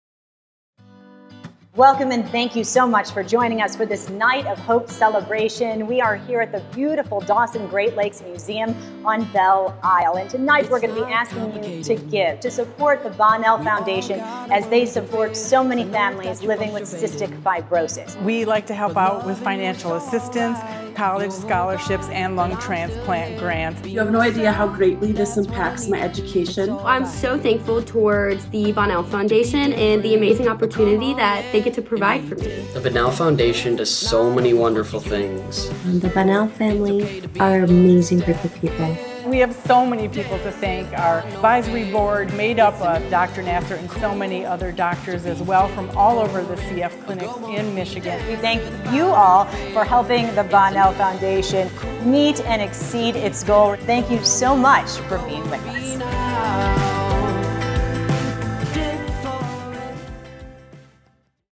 Night of Hope Celebration 2022
This is a one minute snippet of our Night of Hope Celebration.  We can't do what we do without you.  And to everyone who volunteers and participates.  Thank you.